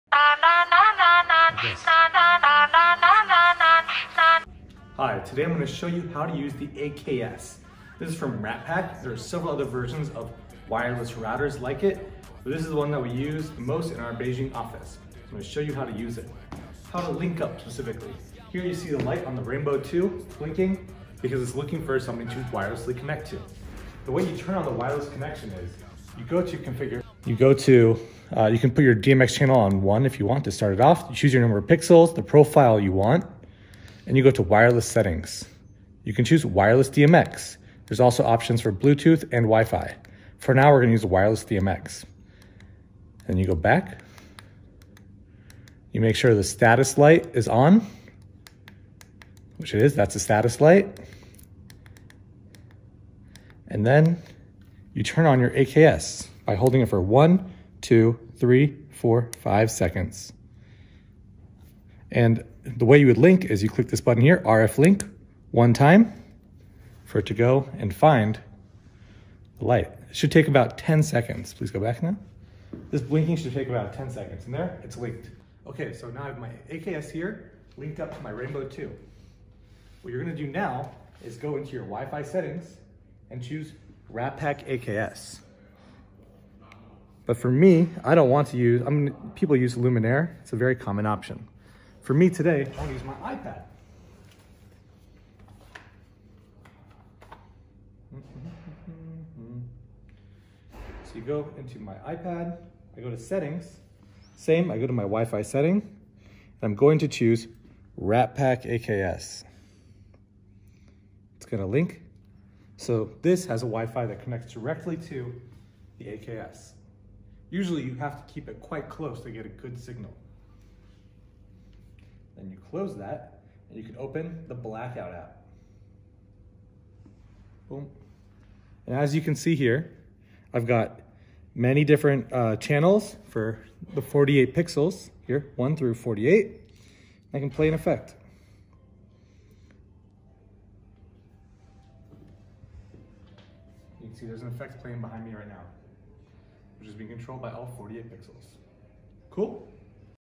How to: Wirelessly link lights to an AKS
ProfessaJ linking the Rainbow 2 to an AKS.
Note from ProfessaJ: "Sorry about looking unkempt. It was 8am and I had to rush this video before a shoot. I cleaned up the space behind me right after as well."